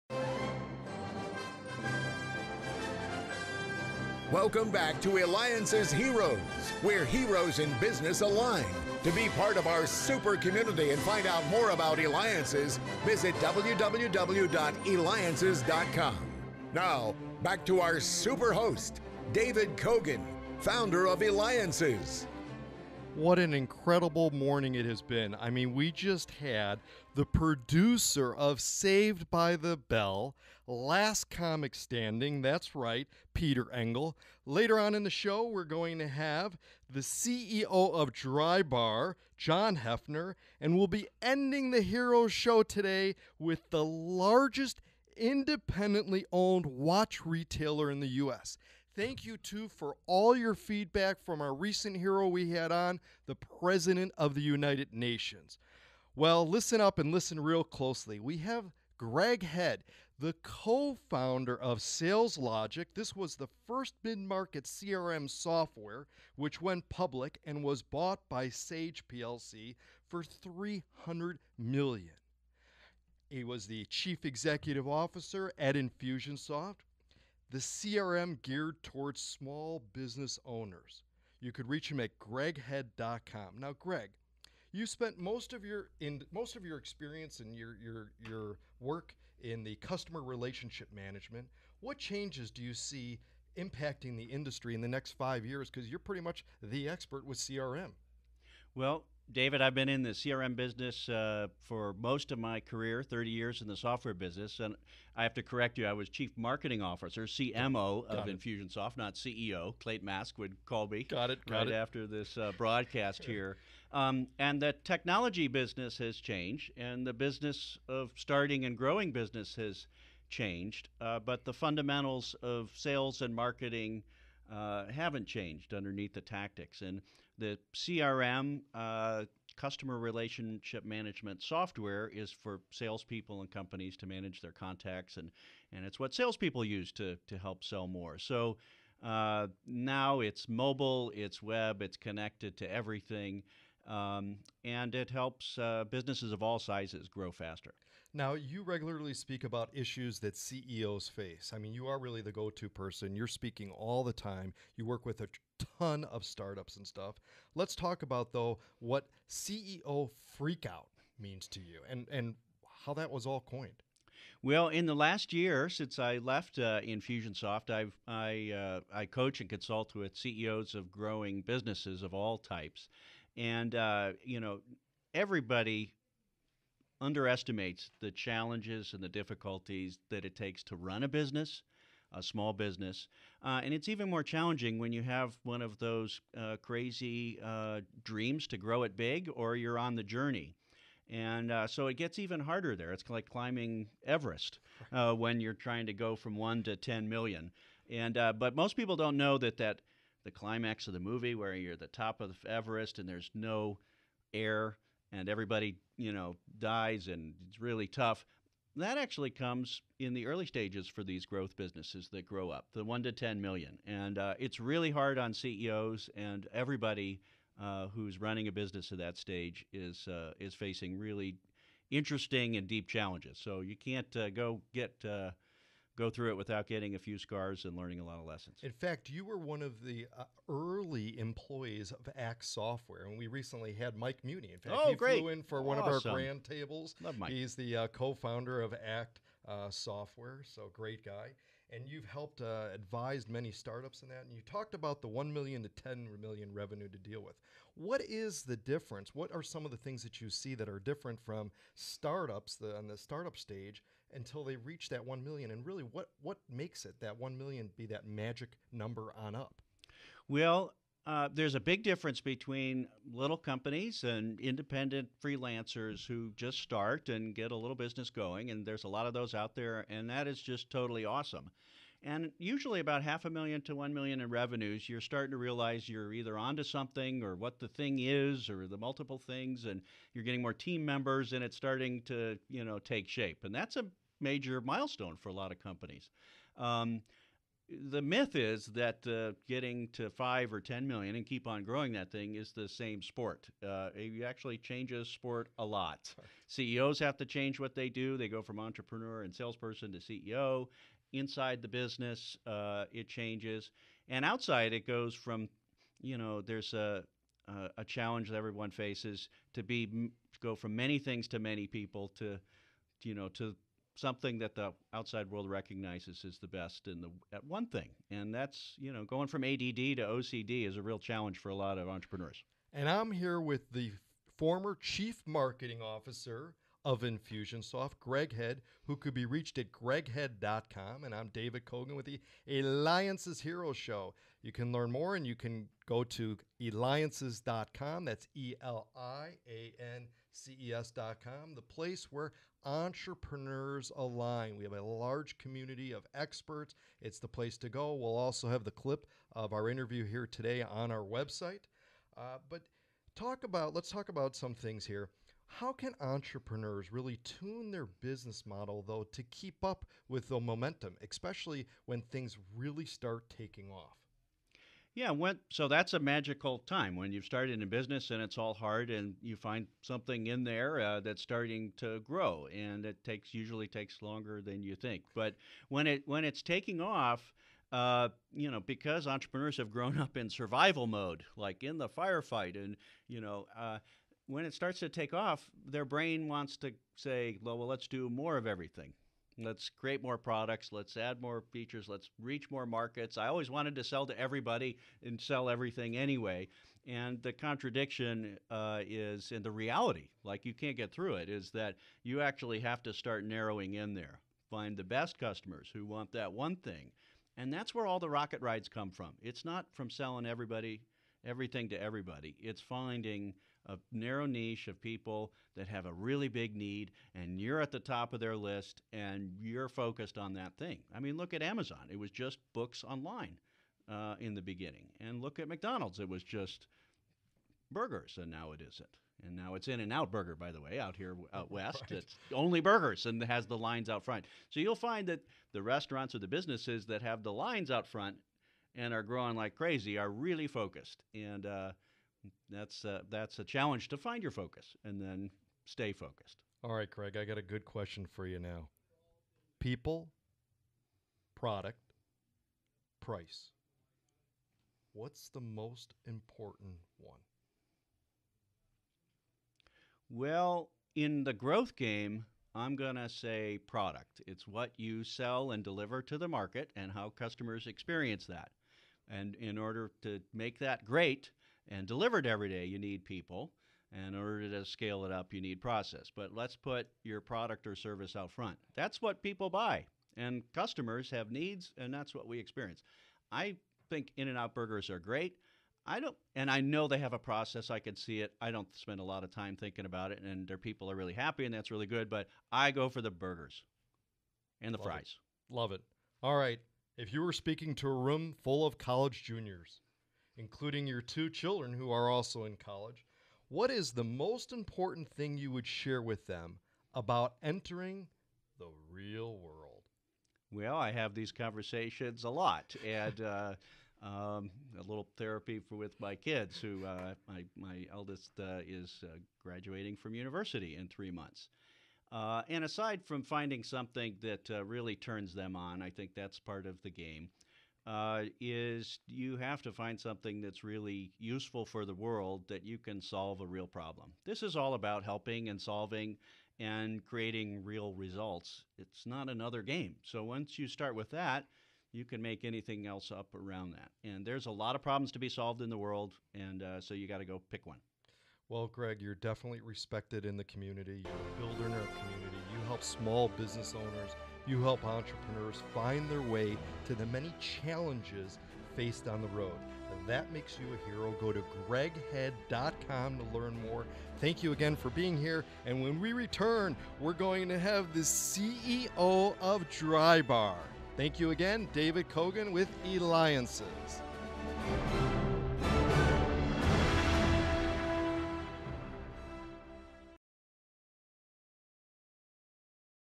Greg Head Co-Founder Saleslogix CRM Pioneer
Greg Head of interviewed by David Cogan of Eliances Heroes.com. Greg and David talk about the early CRM industry wiht ACT! software and Saleslogix of which Greg was a part of both. He later joined another well-known small business CRM offering, Infusionsoft, as Cheif Marketing Officer. Greg now does speaking engagements and coaches business owners about the pitfalls to avoid in running a startup.&nbsp;